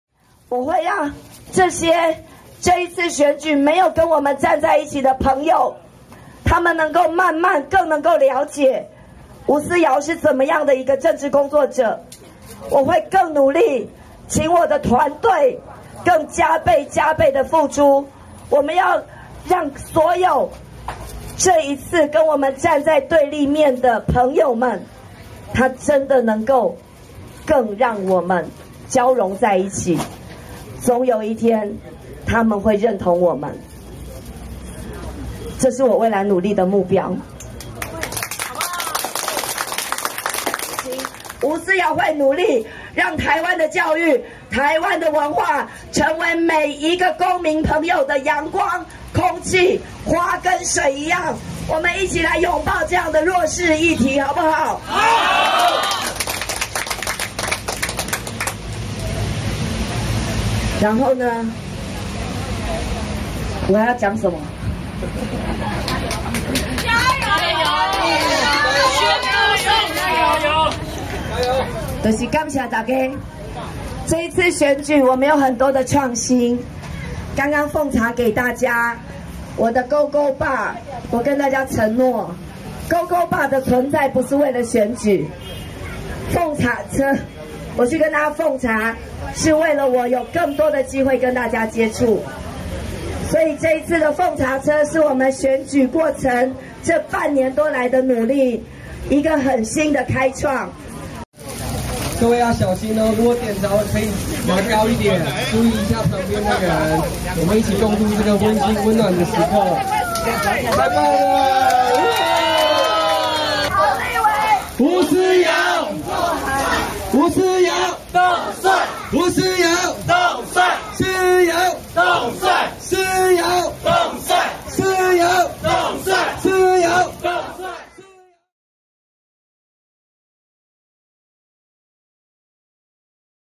順利連任感動落淚 吳思瑤揪支持者點仙女棒慶祝
台北市立委第一選區選戰打得火熱，尋求連任的民進黨立委吳思瑤晚間8點宣布當選，並現身競選總部向支持者表達感謝，激動到落淚，吳思瑤笑說這是喜悅的眼淚，未來她會持續在教育文化領域為台灣發聲，最後吳思瑤與競總支持者齊點仙女棒，慶祝這次順利連任。